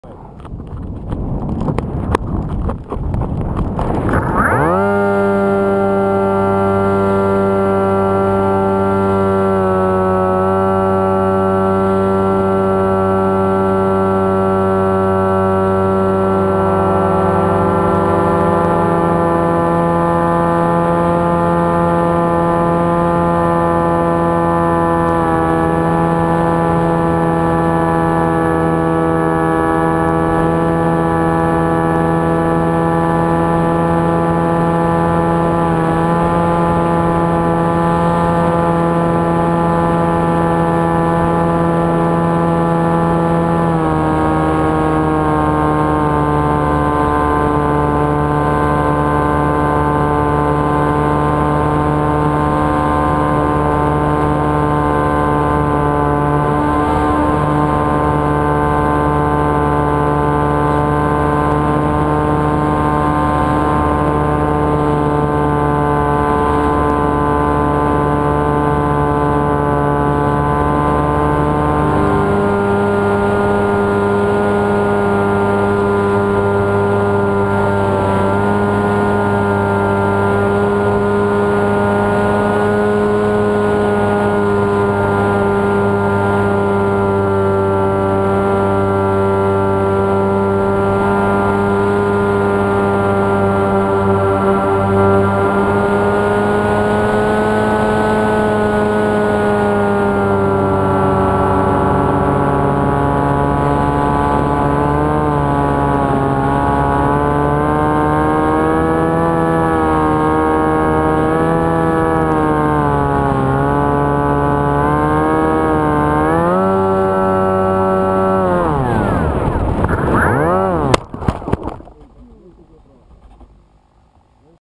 Tudo corre mal... All goes bad... Flow with DragonLink
O que era para ser um voo de grande altitude logo de princípio se revelou cheio de problemas até à aterragem.
What was supposed to be a high altitude flight soon reveled to be full of problems even in landing...